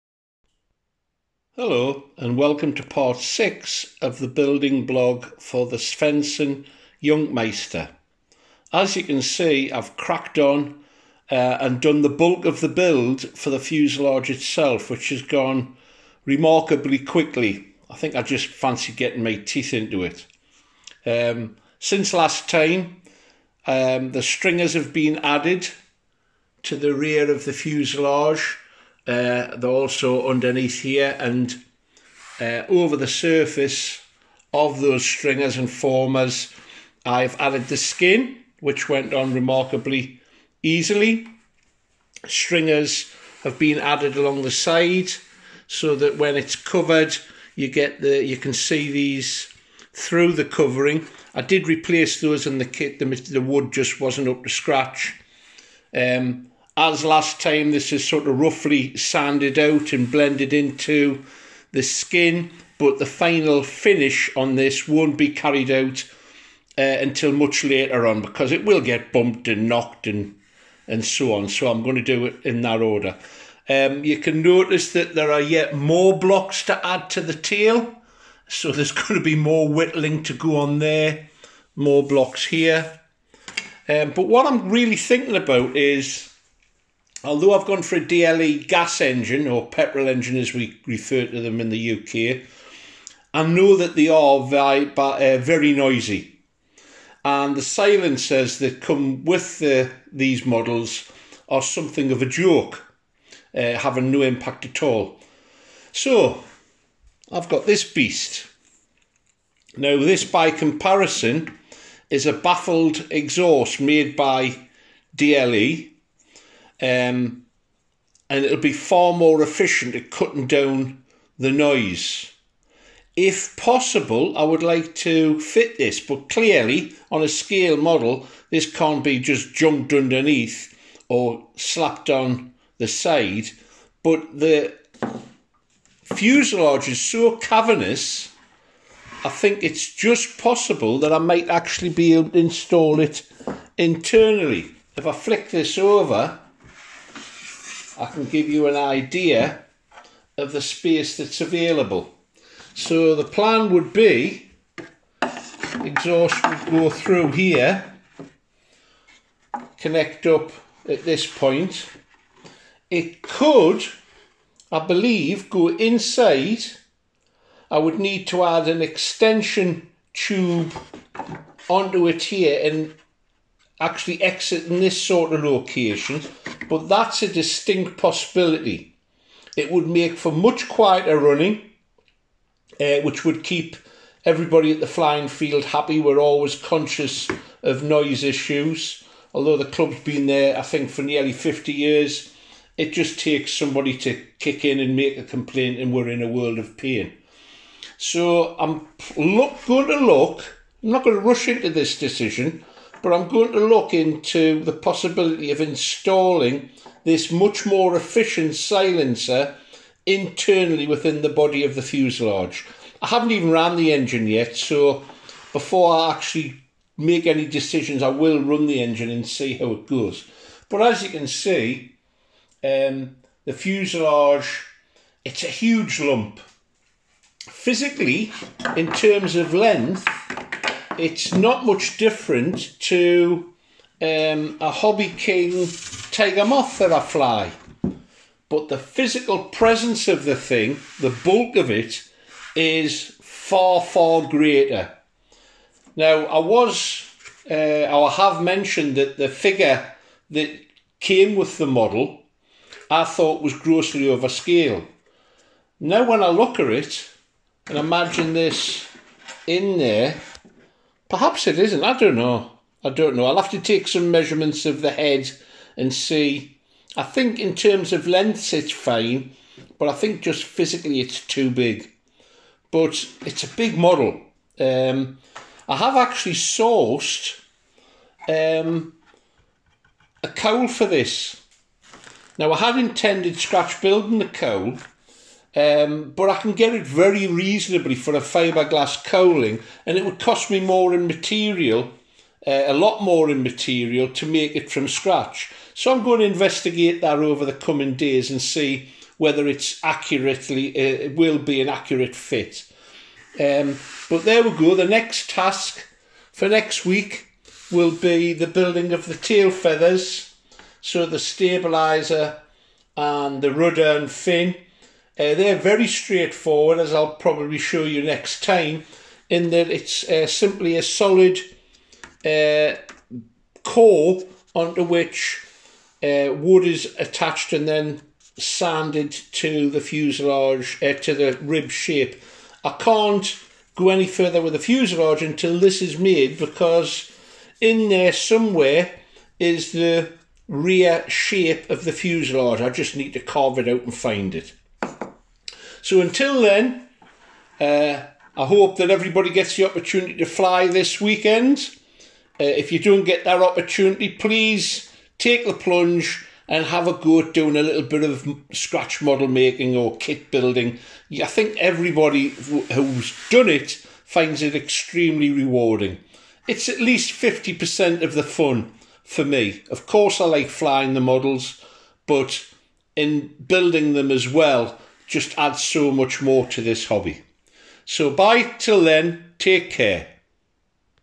Building blog part 6 RC plane: Svenson Jungmeister fuselage.
The bulk of the fuselage build is now done some thoughts on silencer location.